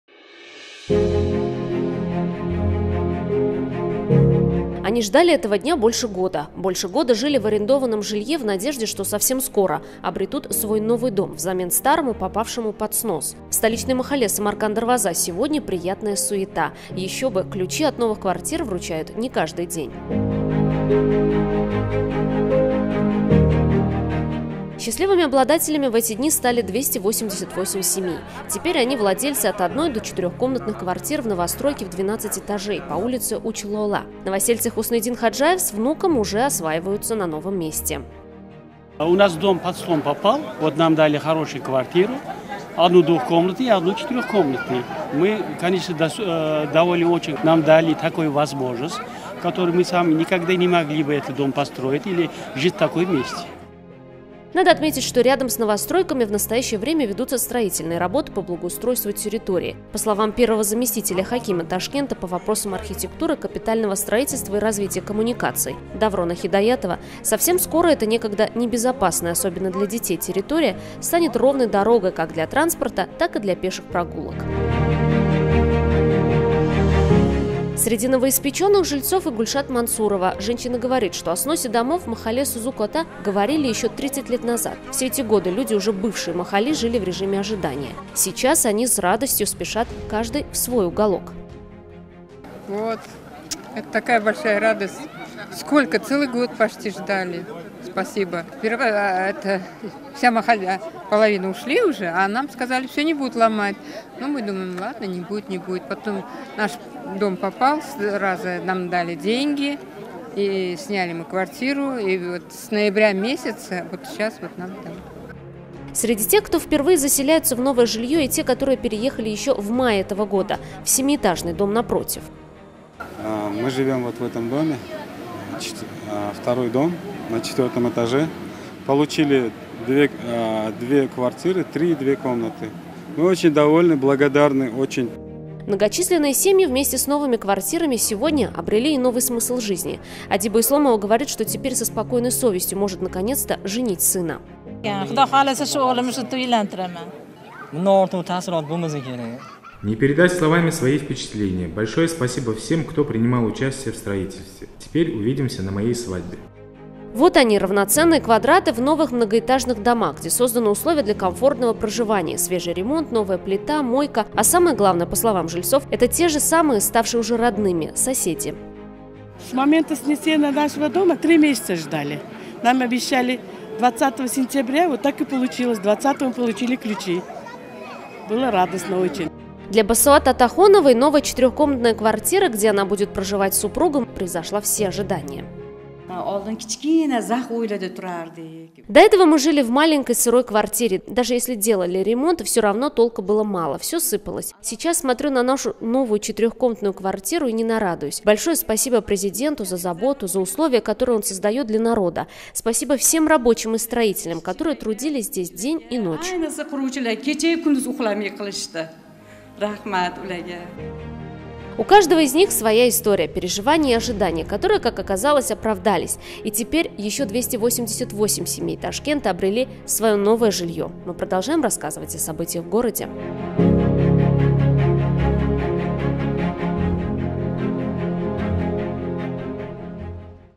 Вручение домов в махалле «Самарканд Дарвоза»
Вчера очередное новоселье справляли 288 семей, получивших ключи от новостройки в махалле «Самарканд Дарвоза».